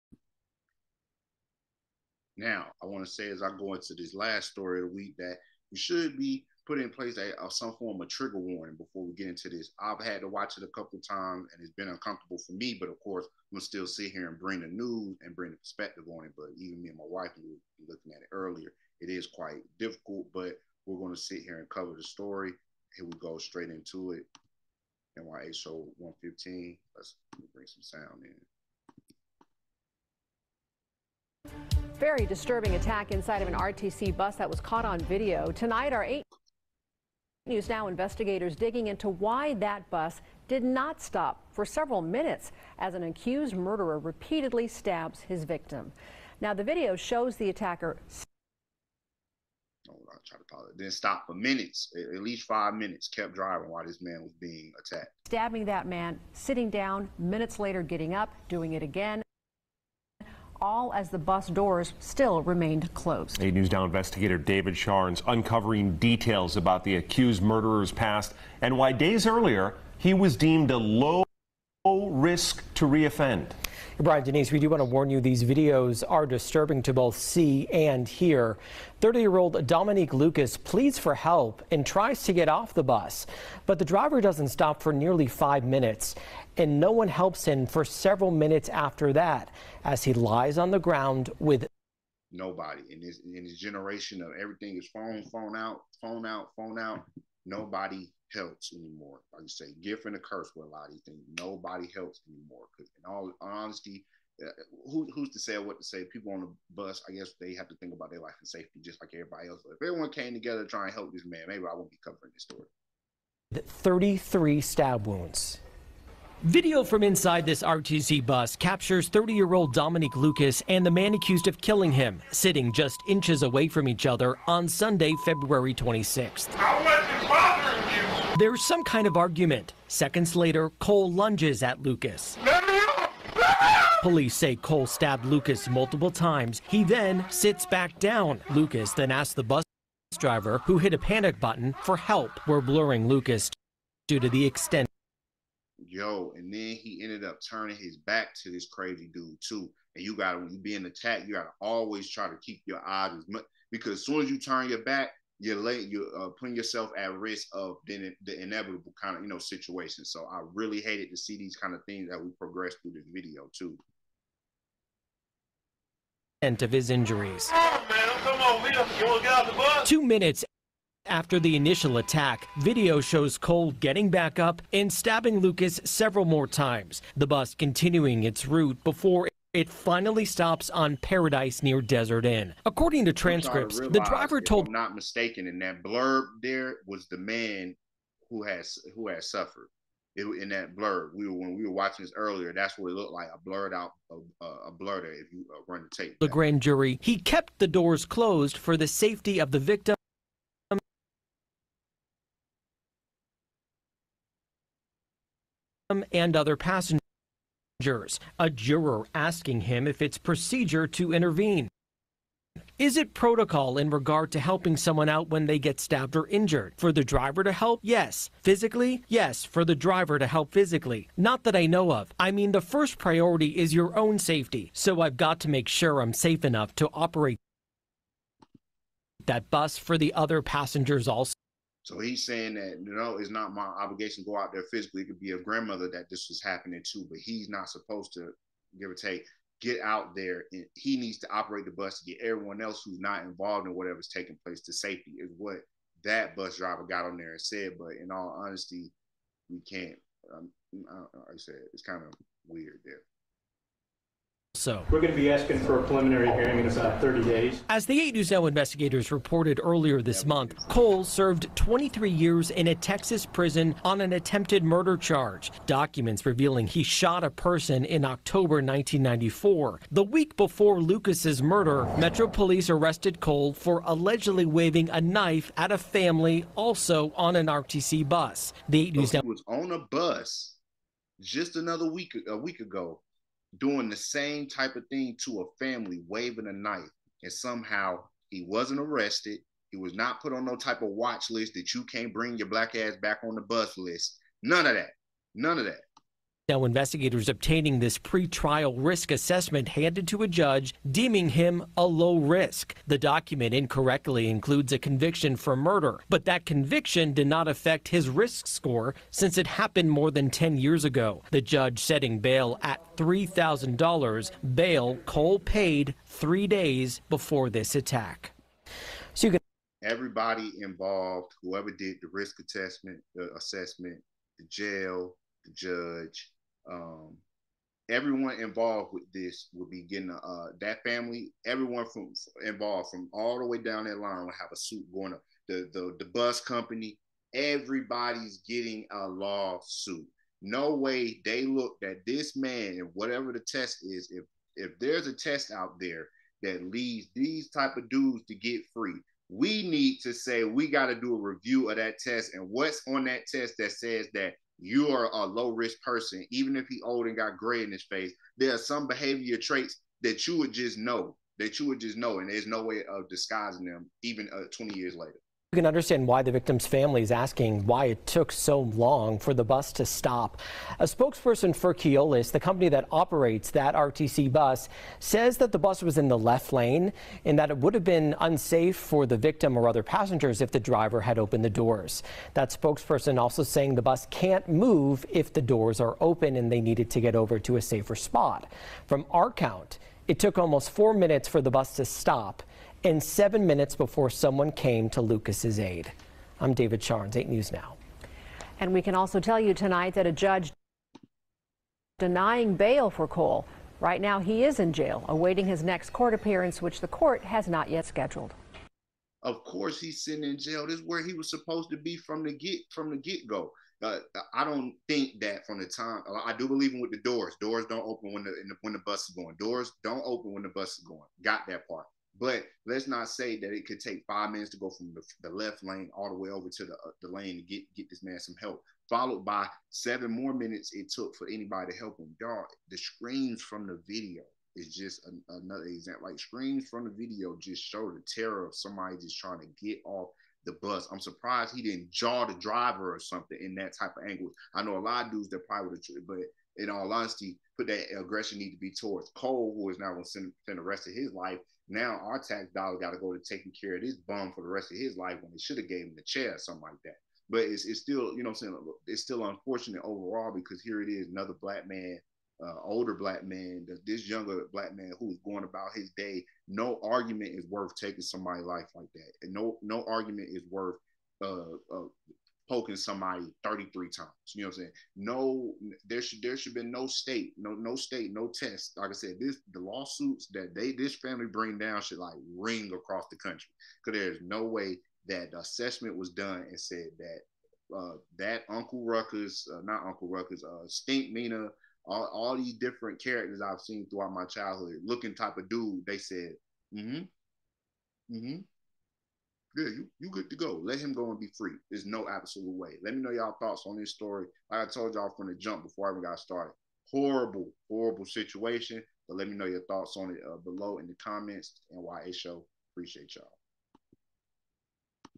Nuthead stabs bus passenger multiple times as driver keeps driving
To Help

One TV: @nya_tko, @neweradetails

New Era Details Photography & Film LLC.